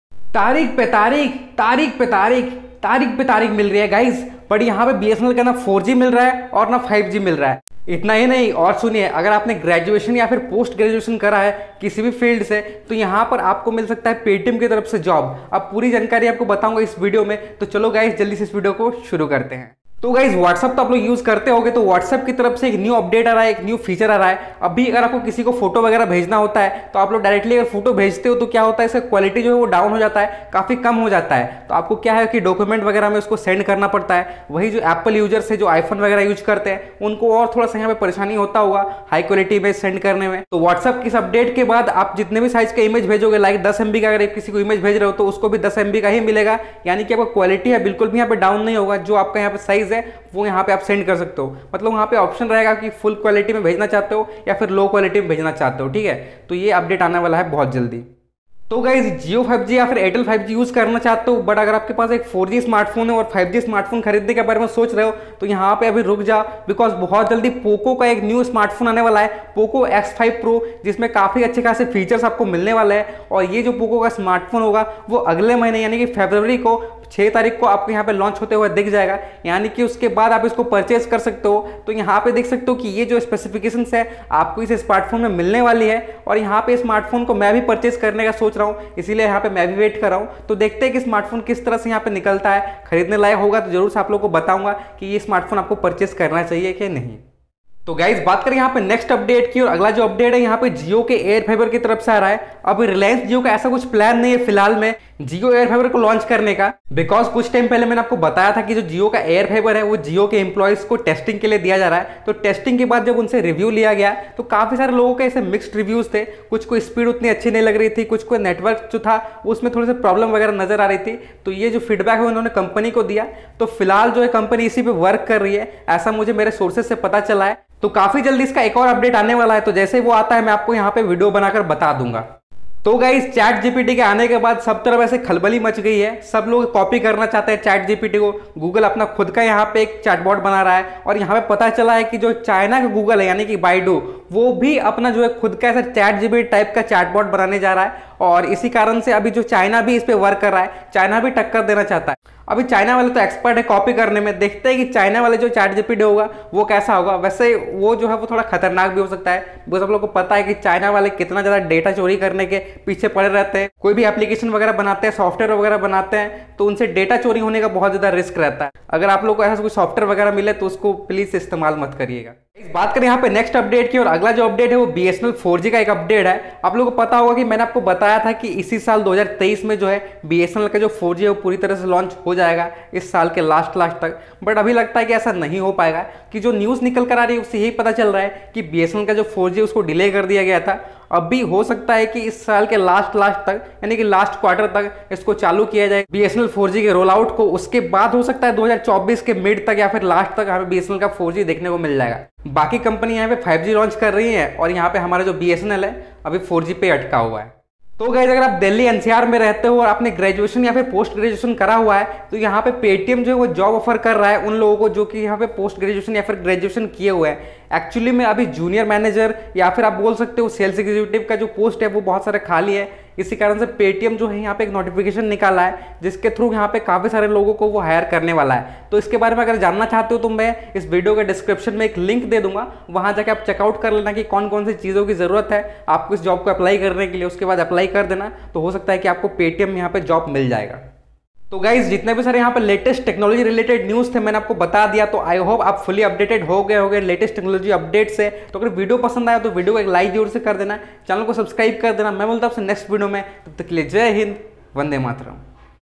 Jio air fiber update, Bsnl 4g launch date, Paytm job offer, Whatsapp new feature, China's ChatGPT
Hey guys, in this video I am going to tell you some of the latest technology related news/updates.

1) jio air fiber 5g update
2) Paytm job offer notification
3) China's chat gpt
4) Bsnl 4g latest news
5) Whatsapp upcoming features
6) ChatGPT AI
7) latest tech news
8) Tech news today
9) Technology updates today